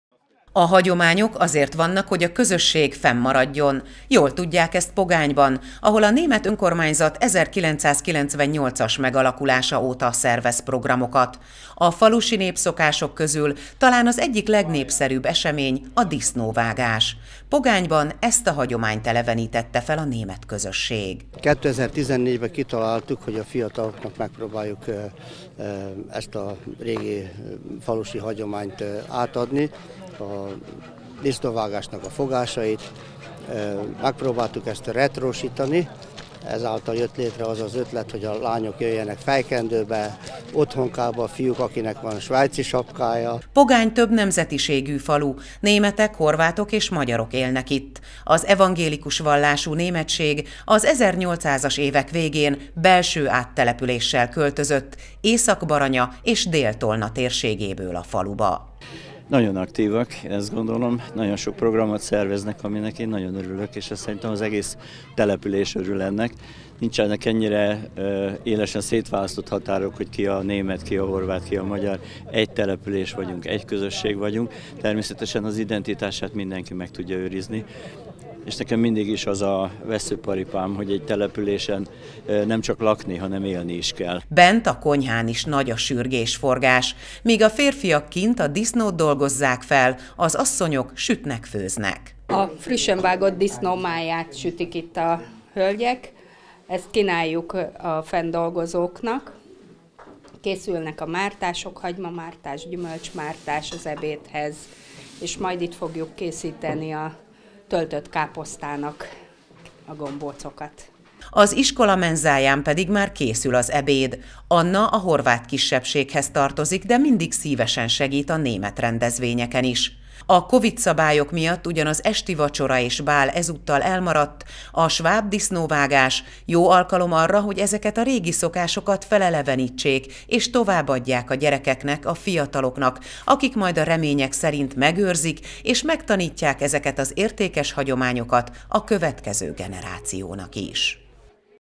Sváb disznóvágás
Hagyományőrző sváb disznóvágást tartottak Pogányban. A cél, hogy a településen élő fiatalok is megismerjék a sváb hagyományokat. A rendezvényen a szervező hölgyek kötényt, a férfiak pedig svájci sapkát viseltek. A covid-szabályok miatt az esti vacsora és bál ezúttal elmaradt.